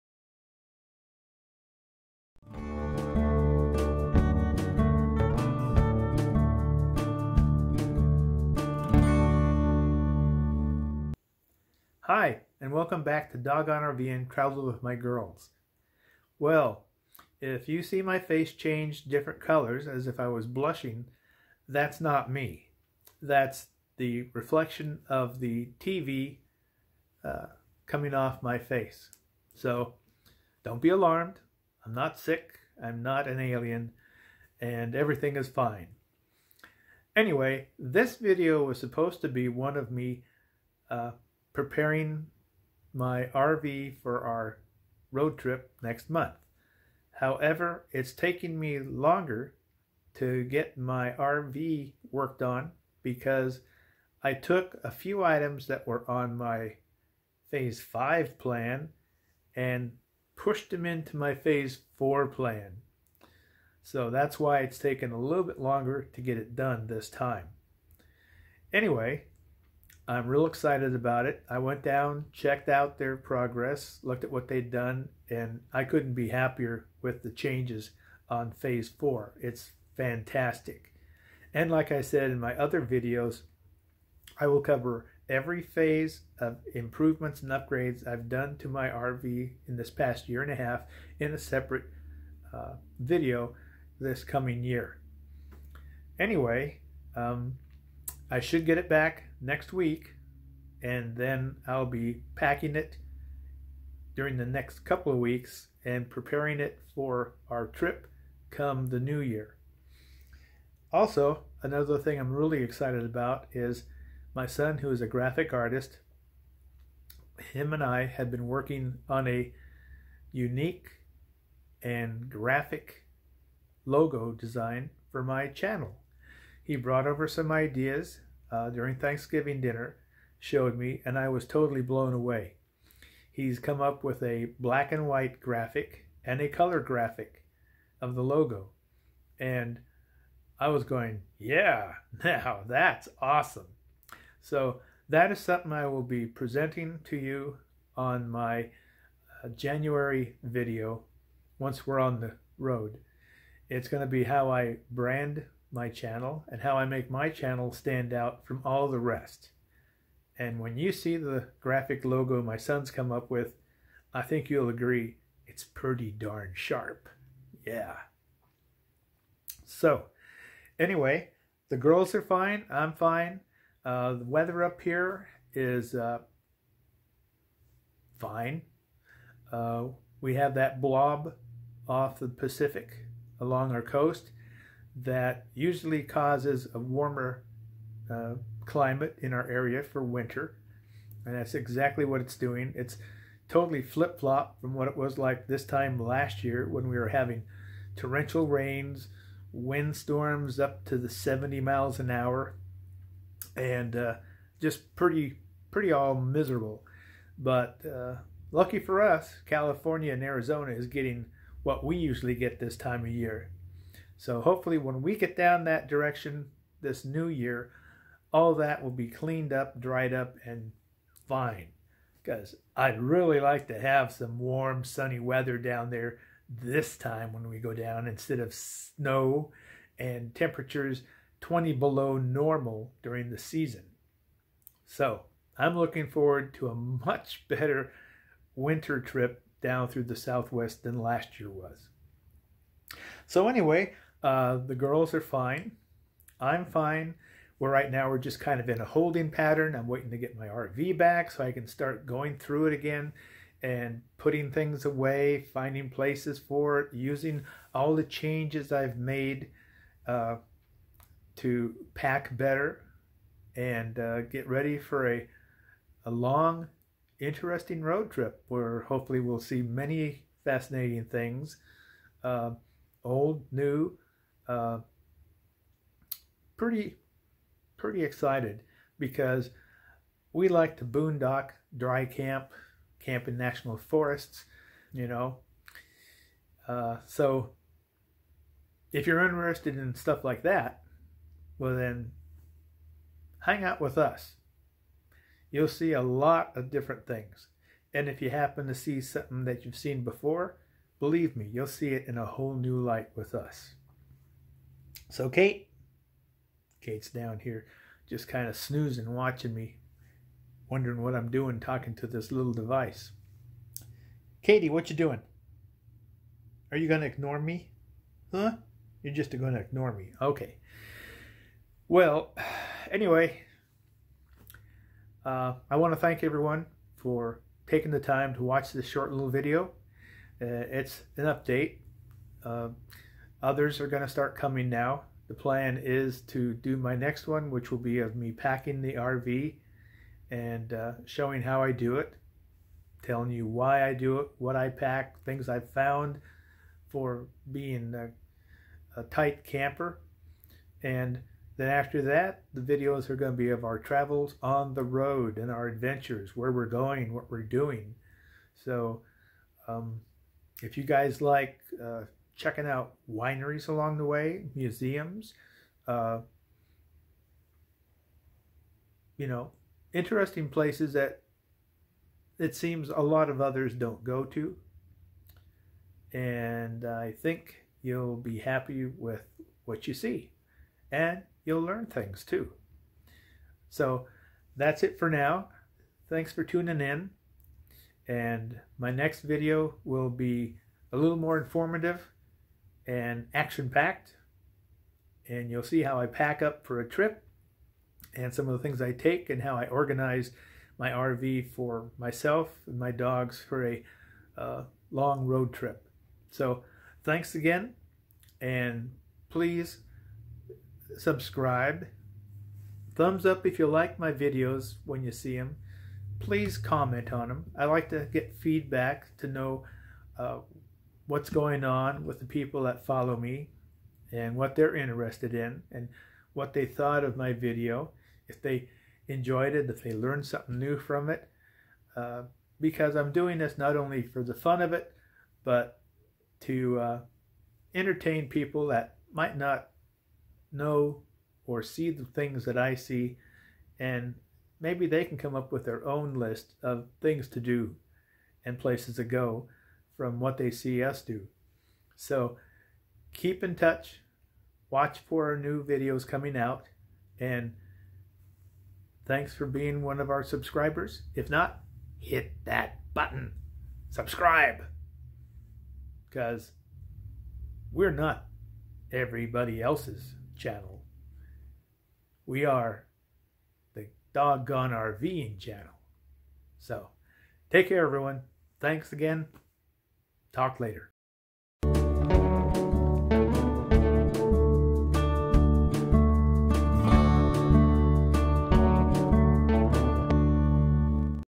RV UPGRADE UPDATE
I slid a few ideals from my future planed  Phase V  Campervan improvements into my current  "in work"  Phase IV Campervan  improvements while my rig is currently in the shop,  so it is taking a little longer to get my RV back  this time!  Meanwhile my Graphic Artist son is working on a logo for me to "brand"  my You Tube channel like others have theirs,  to stand out in the crowded You Tube Universe.
         Time is counting down till the girls and I  hit the road again - finally! All three of us are more than  eager to get out of this continual rainy, gray,  damp and cold PNW winter weather !  The energy in my videos will  "pick up" after this update video with my next one kicking off  the start of our 2020 travels.  We are EXCITED  to GO!
       Arf! Arf!